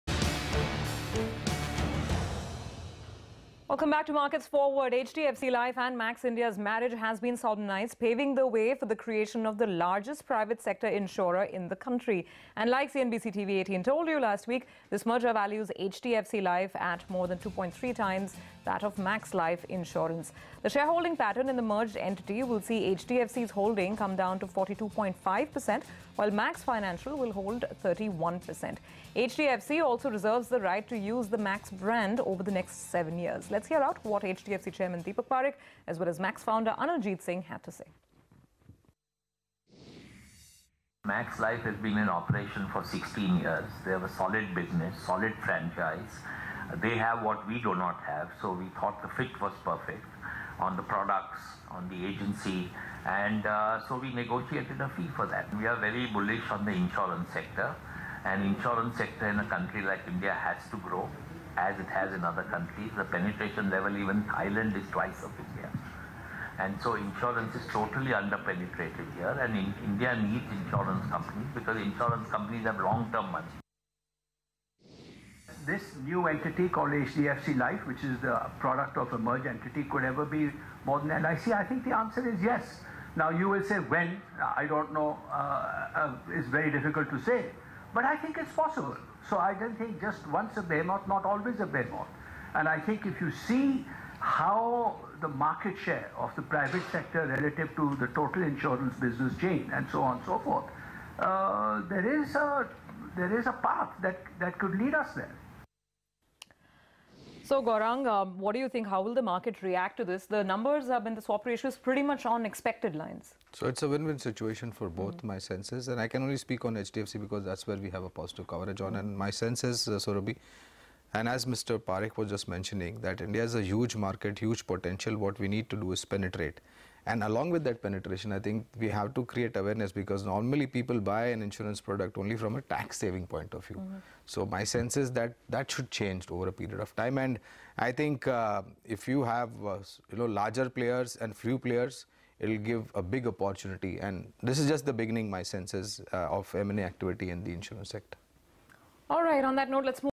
HDFC Life, Max Insurance: A Done Deal. Building India's Largest Private Sector Insurer.
HDFC Life and Max India's marriage has been solemnised paving the way for the creation of the largest private sector insurer in the country. Listen what the top management and experts have to say on the merger deal here.